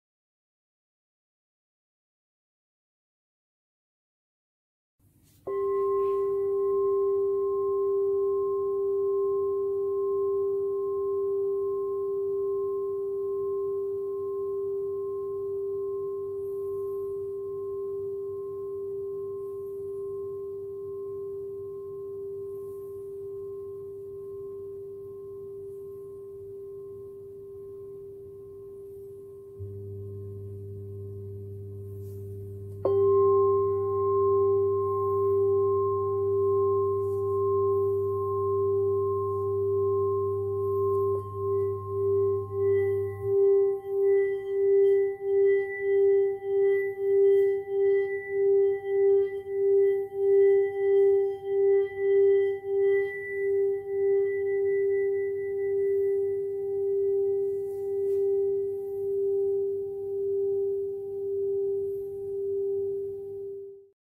黃金菊水晶聖杯｜鍍金菊花｜人工打磨｜喉輪｜G調｜5.5寸｜Rudbeckia Crystal Singing Cup｜Handmade｜Throat Chakra｜Gilded｜G Key
你也快點帶一個回家吧, 有關產品更多詳細資料，請瀏覽以下鏈結
Bring one home and enjoy the vibration today! For more product details, please see the